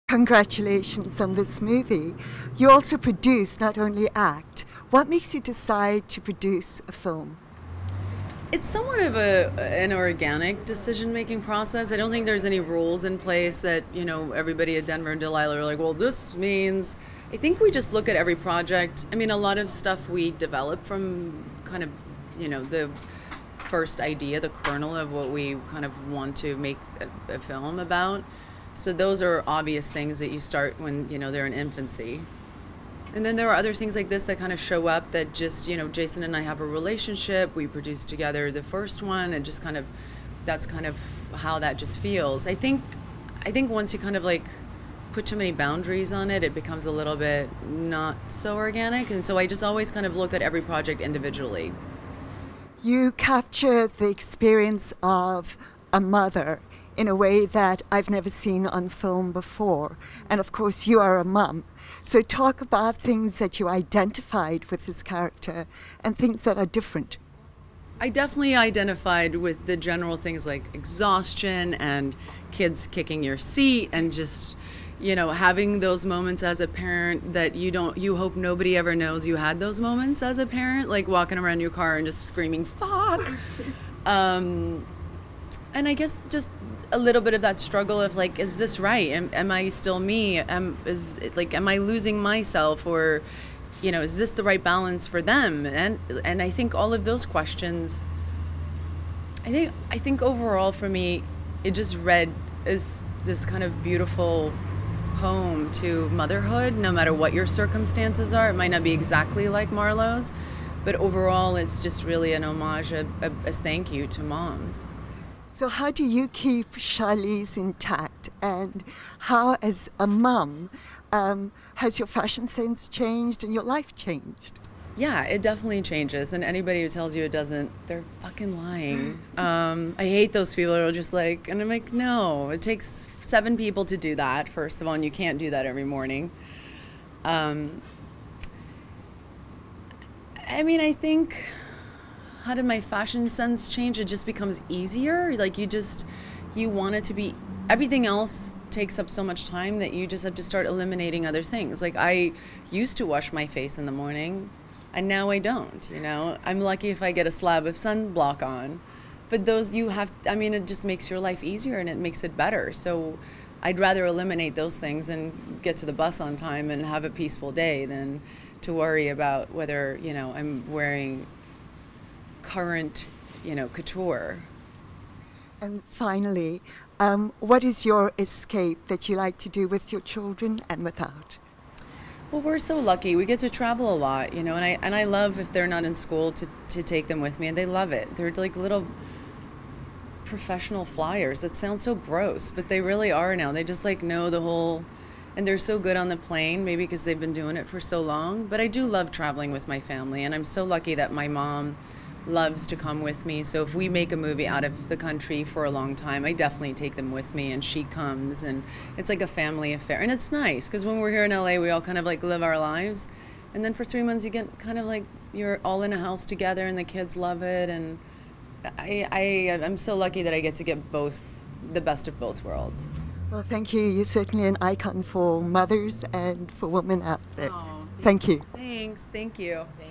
5 Minutes With Charlize Theron On Her New Film 'Tully'
We catch up with Charlize Theron to chat about her new movie 'Tully', motherhood and what she does for her personal escape.